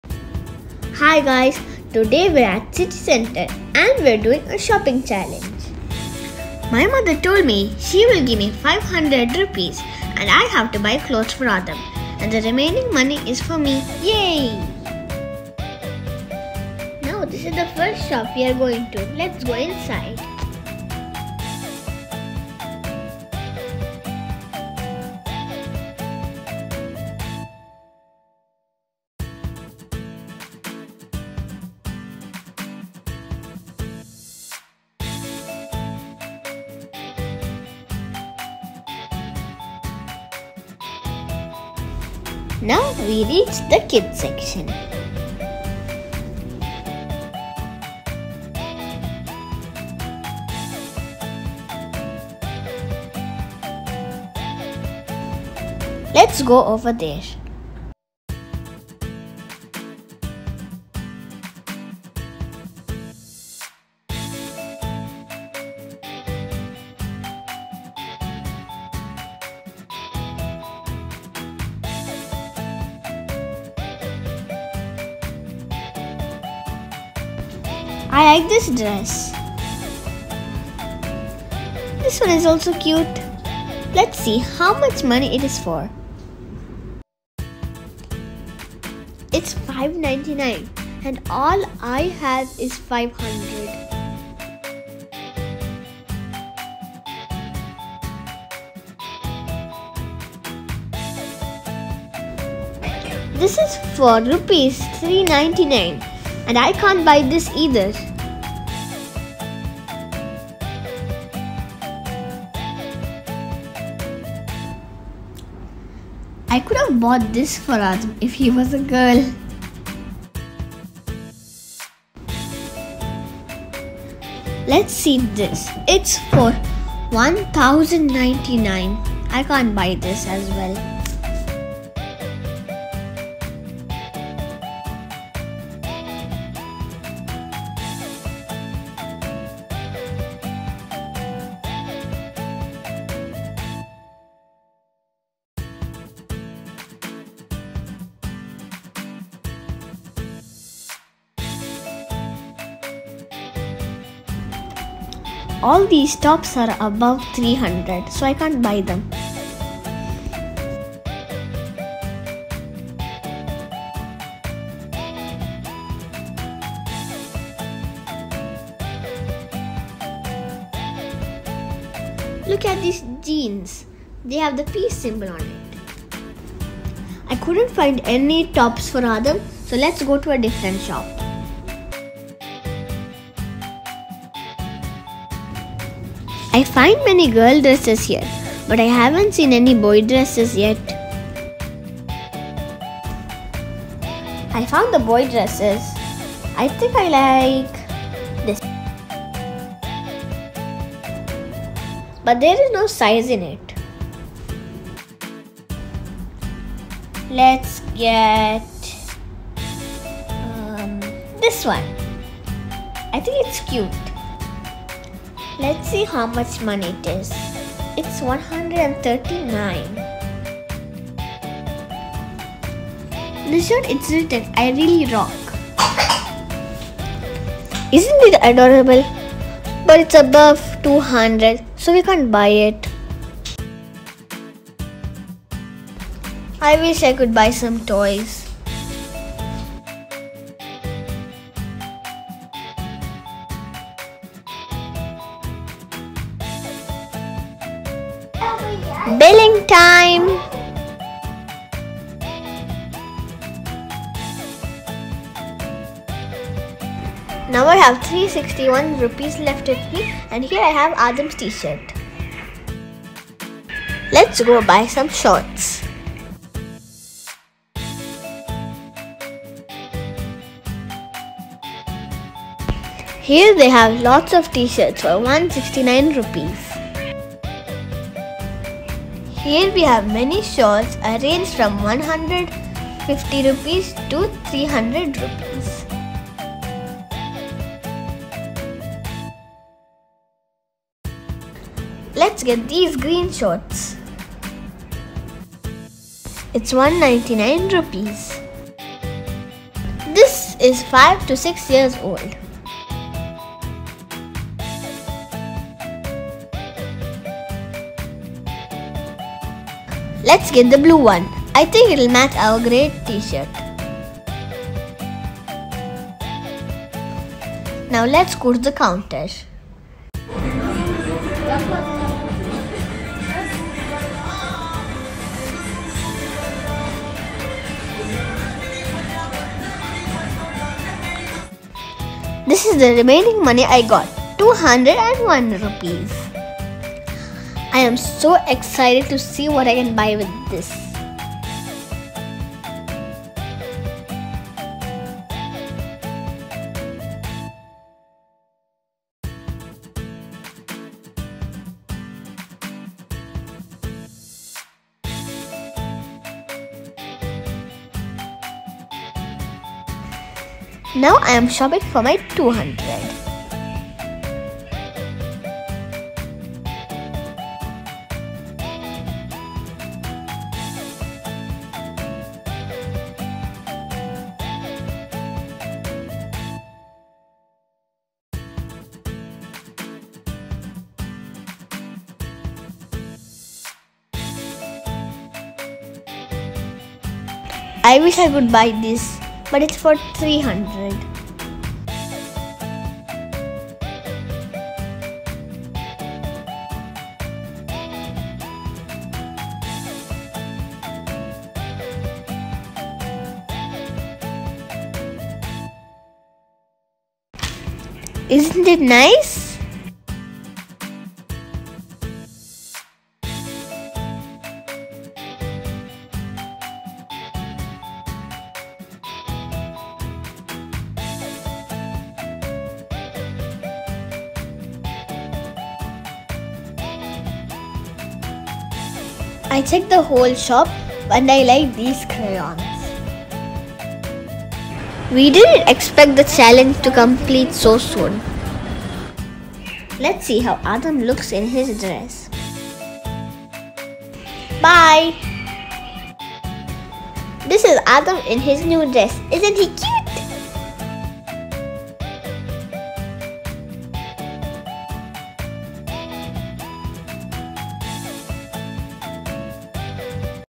Shopping Challenge@CC Mangalore|Emma And Her Unicorns
Shopping challenge!!

My maami gave me ₹500 and challenged me to buy an outfit for Adam, my lil bro. The fun part was, the money I save is my pocket money.🙇🏻‍♀️

We are gonna take a short break. But we will be on Instagram. Please Follow us there